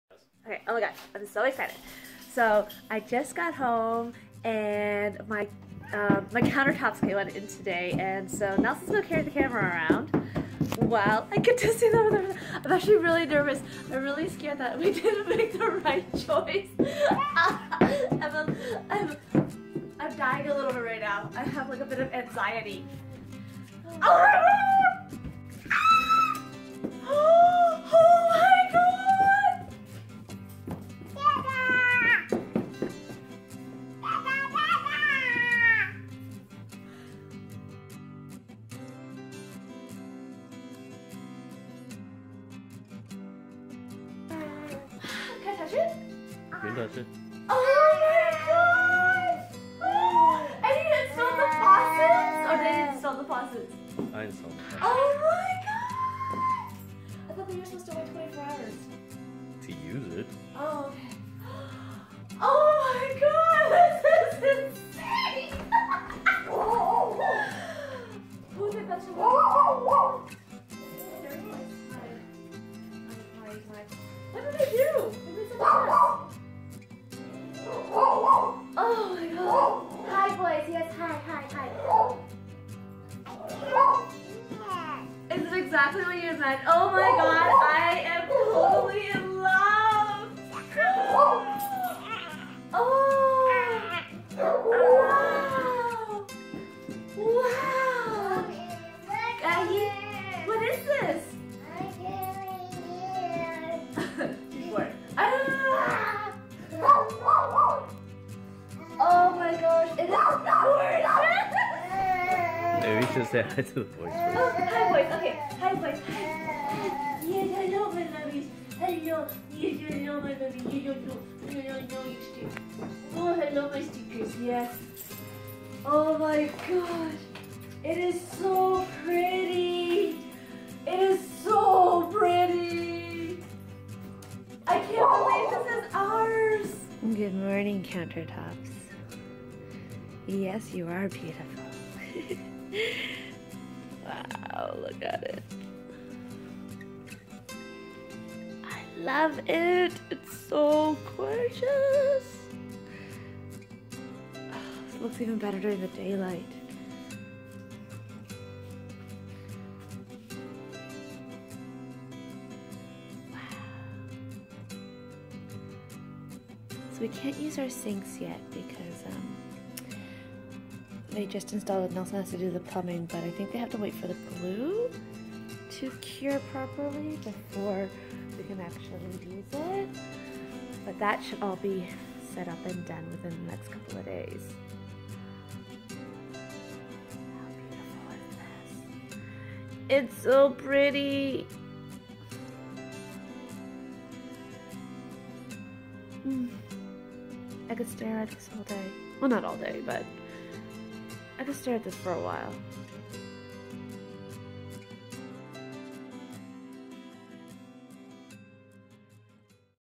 Reaction Video! New Countertops are here!
OMG OMG OMG  You don't understand HOW excited I was that day!! It's been a year and a half since we started our kitchen reno project and finally...FINALLY the countertops were installed!!  I was so excited all day and I kept telling the girls at the studio that they're coming in!! They're coming in! Haha

Please ignore the messy background as everything else is still undergoing renos and we had to move things quickly out of the way for the fabricators to move the countertops in.

Also! Disclaimer that we did not use a gimbo or tripod...so you're in for a bumpy ride.  Gravol or ginger recommended for nausea.

xo's

Jessie